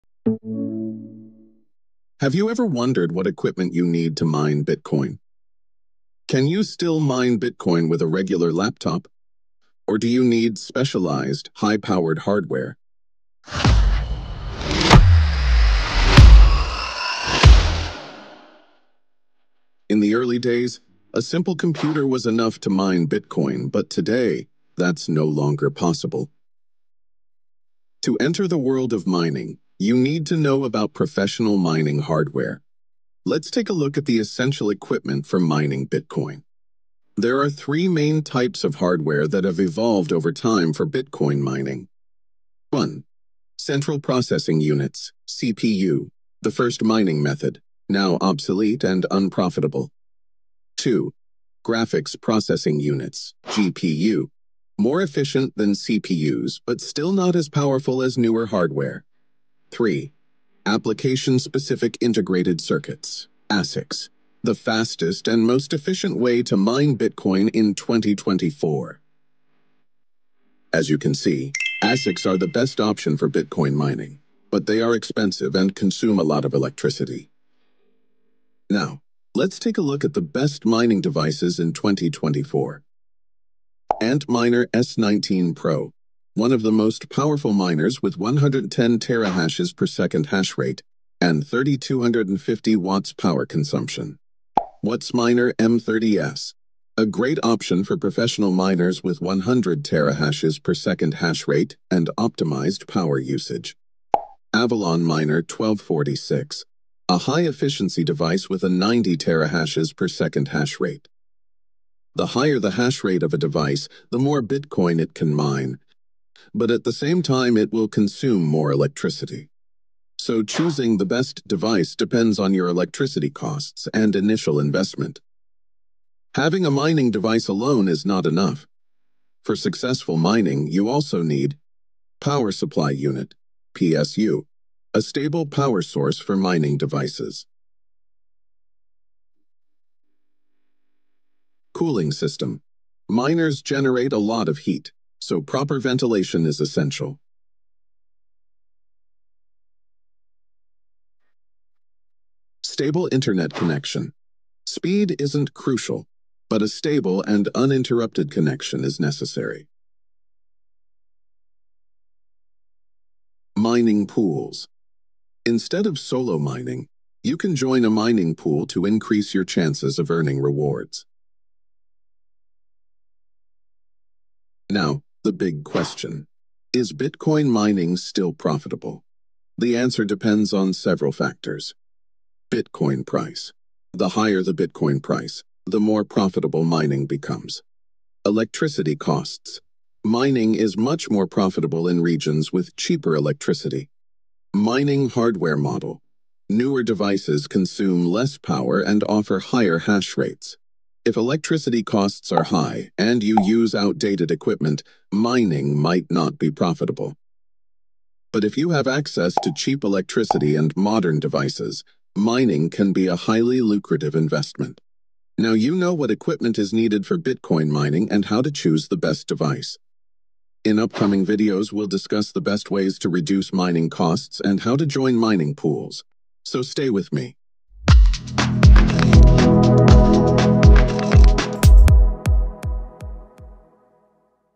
📢 Bitcoin Mining Equipment & Hardware – Which Devices Offer the Best Performance? ⛏️💻
💡 Do you know what hardware is required for Bitcoin mining? 🤔 Can you still mine Bitcoin with a home computer, or do you need professional equipment to stay competitive?

🔍 In this video, you’ll learn about the best mining equipment in 2024:
✅ Different types of mining devices – CPU, GPU, and ASIC ⚙️
✅ Comparison of various devices and the best options for Bitcoin mining 📊
✅ Which ASIC models are the most profitable right now? 💰
✅ How much power do these devices consume, and what are the electricity costs? ⚡️
✅ Strategies to improve efficiency and reduce mining costs 🔥

🎬 If you’re planning to enter the world of Bitcoin mining, don’t miss this video!

📌 What do you think? Which mining device do you recommend?

🔗 Join now and be part of this success!